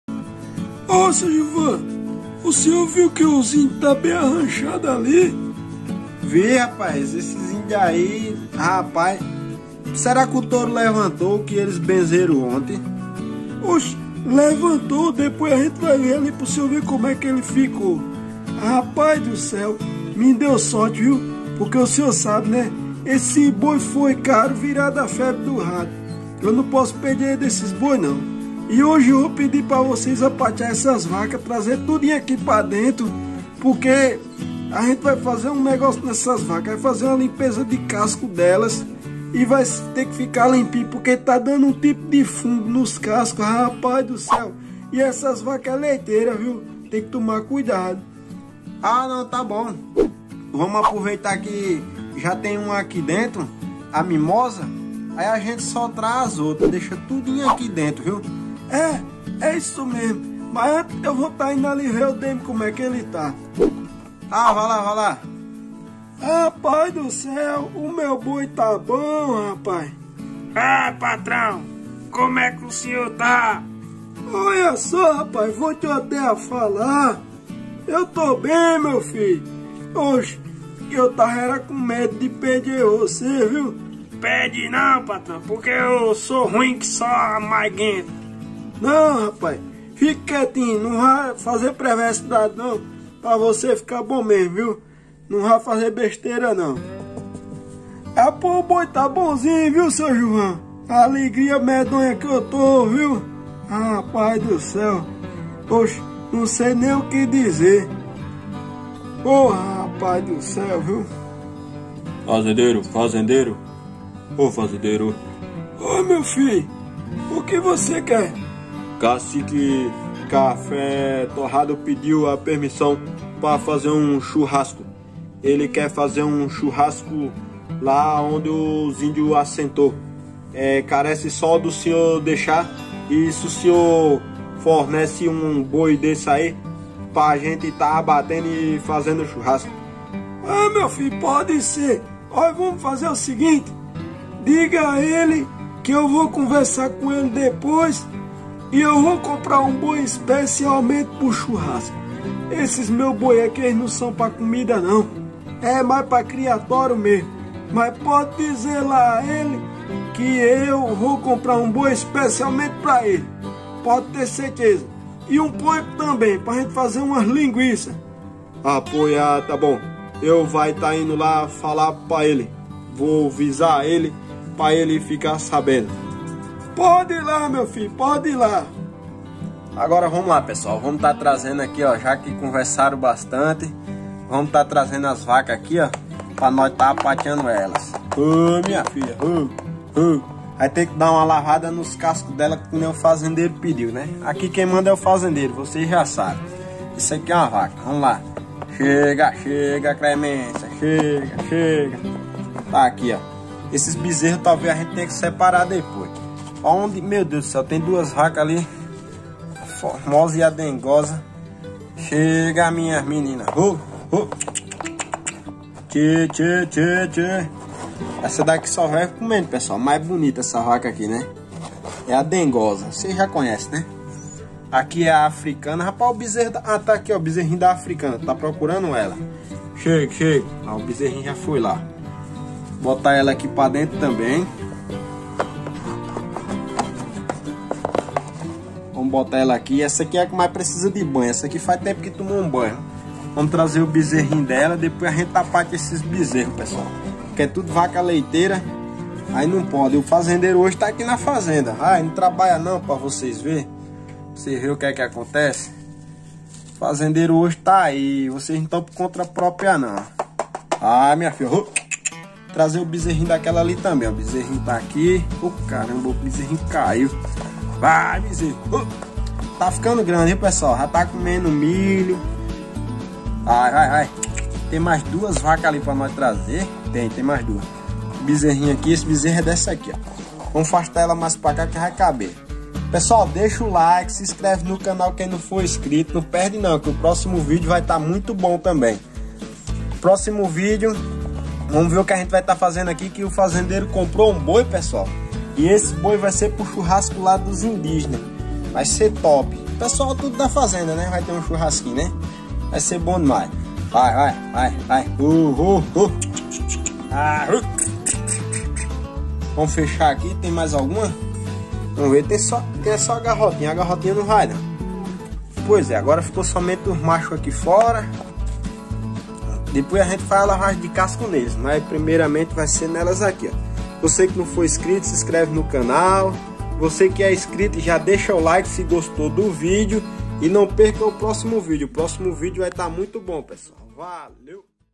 fazenda de brinquedo prendendo as vacas no cural
levei  todas as vacas para o curral para limpeza de cascos na fazendinha de brinquedo
nossas vacas estavam todas sujas com muita poeira nelas, e hoje resolvemos fazer uma limpeza de cascos em todas as vacas da fazenda, não perca o nosso vídeos está muito bom, depois iremos deixar as vacas em um cural mais confortável.